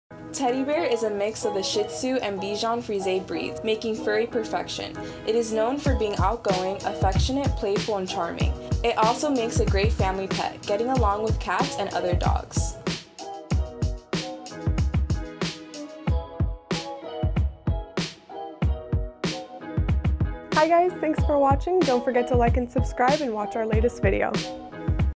Meet the Teddy Bear | Petland Florida
The TeddyBear, the cute combination between a ShihTzu and a BichonFrise, may be the PAW-fect furry friend for you! 🐶😊